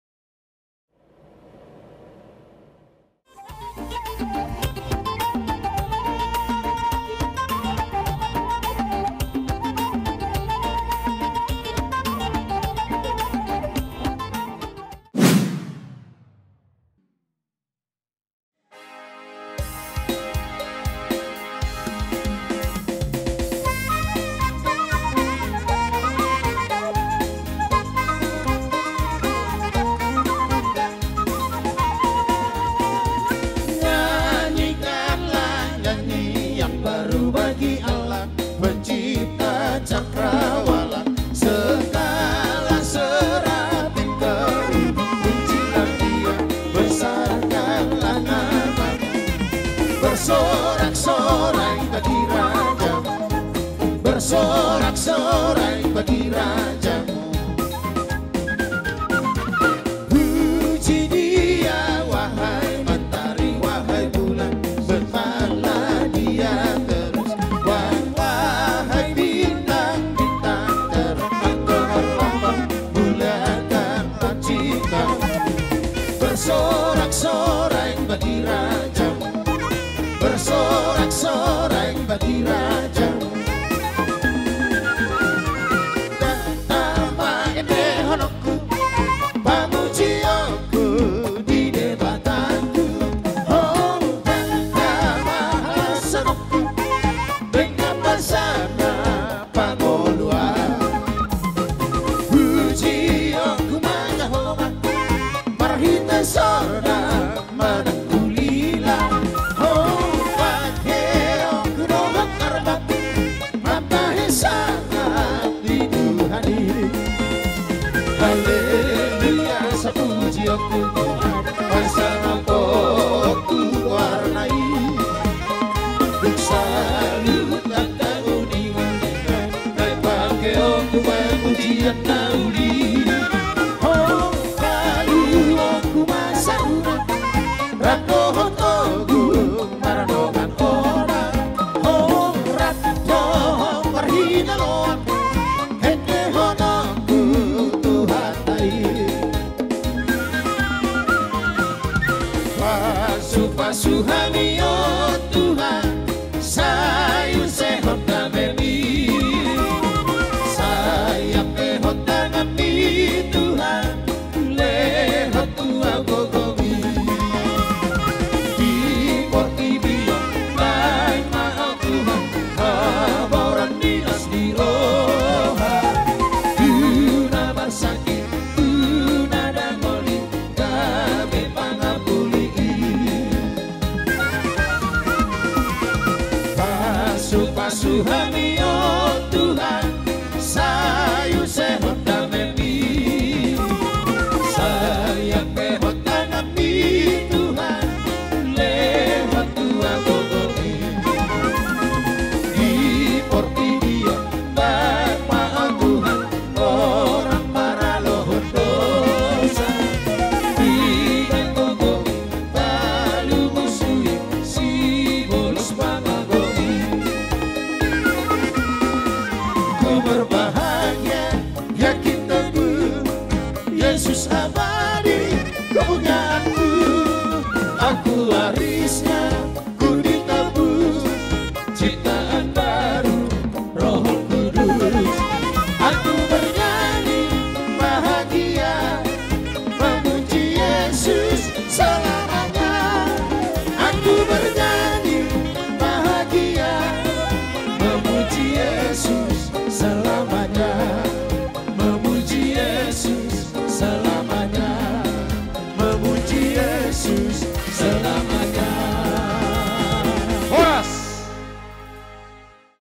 PROSESI MASUK PENGANTIN | Renaldo Gultom & Elisabet Boru Siahaan | Graha Delima
Booking Now ❗️❗️❗️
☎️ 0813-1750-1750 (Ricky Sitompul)